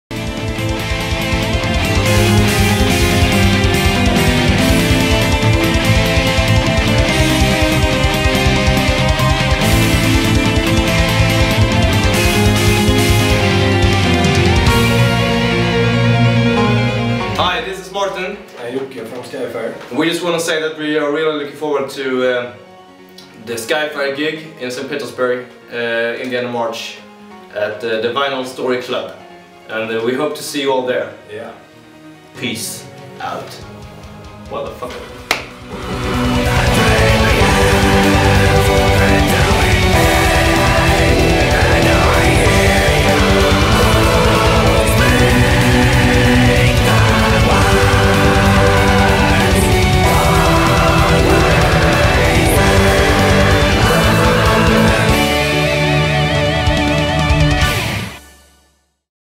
Skyfire SPB promo
Vinyl Story Club ST PETERSBURG и Mikhail Schteinberg & Co представляют:
Это будет 30 марта. Группа SKYFIRE (melodic-death-metal, Sweden)